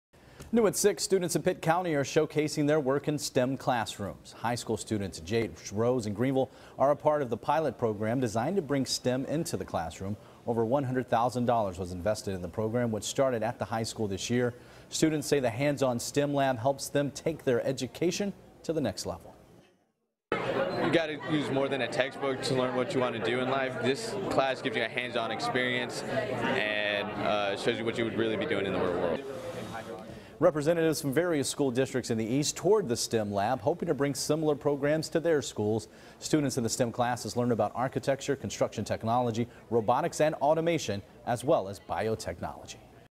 STEM Lab News
CBS News Story: Learn about the Paxton/Patterson STEM program in Pitt County Schools. Students say the program helps them take their education to the next level.